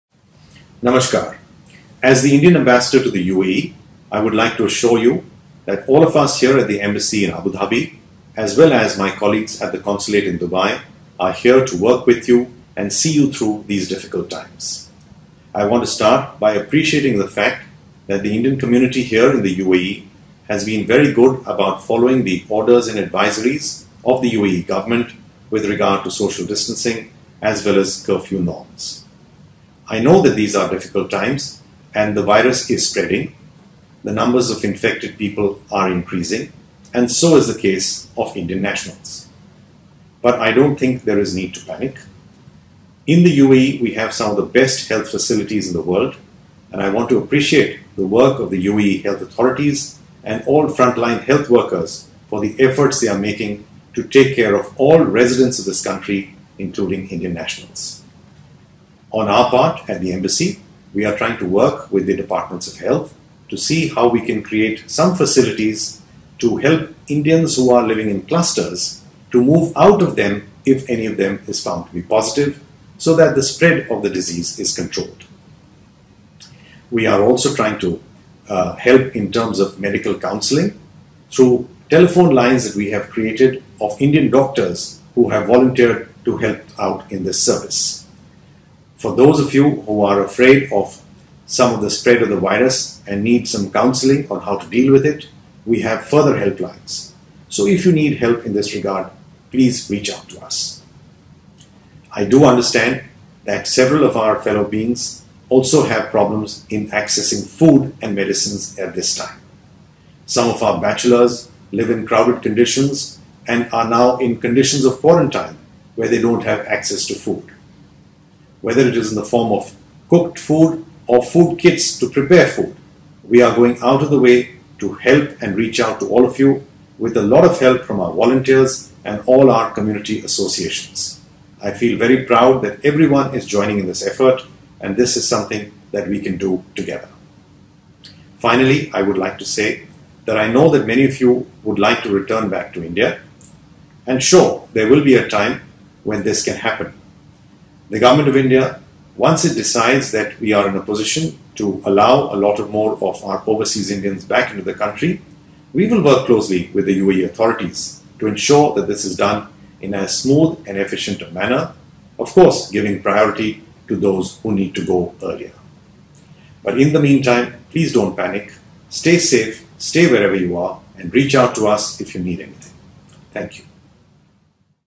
Message of Ambassador Pavan Kapoor to Indian nationals in the UAE on COVID-19
Message of Ambassador Pavan Kapoor to Indian nationals in the UAE. Our team of doctors will call you back.